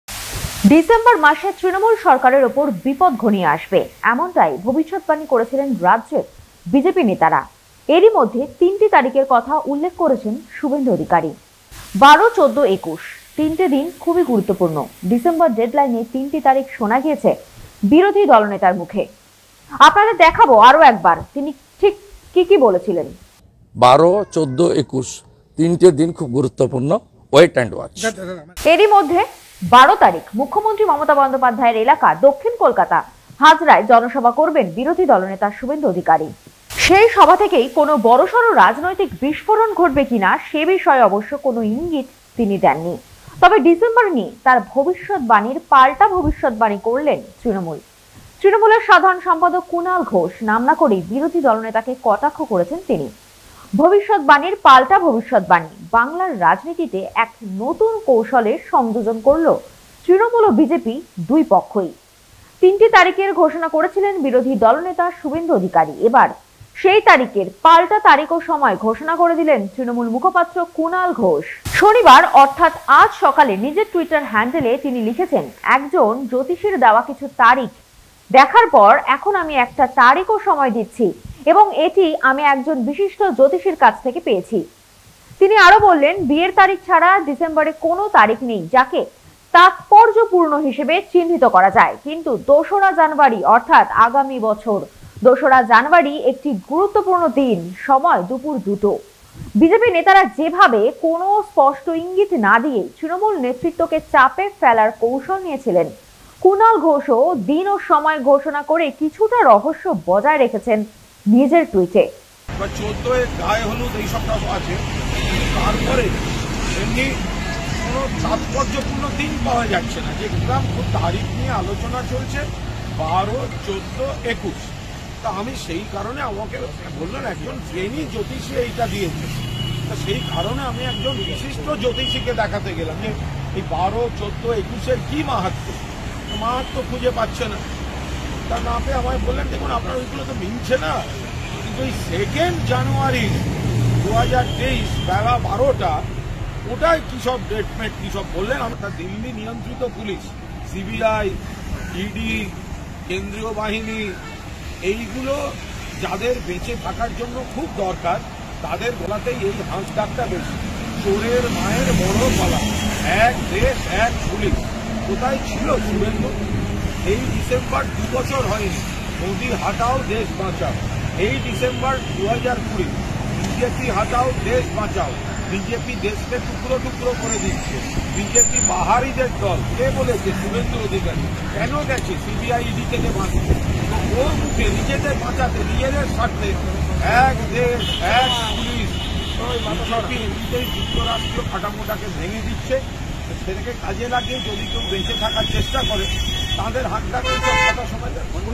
ডিসেম্বর বনাম জানুয়ারি! শুভেন্দুর 'দিন' ঘোষণার পর এবার তৃণমূল মুখপাত্র কুনাল ঘোষের পাল্টা দিন ঘোষণা
ভবিষ্যদ্বাণীর পাল্টা ভবিষ্যদ্বাণী! ডিসেম্বর বনাম জানুয়ারি! শুভেন্দুর 'দিন' ঘোষণার পর এবার  তৃণমূল মুখপাত্র কুনাল ঘোষের পাল্টা 'দিন' ঘোষণা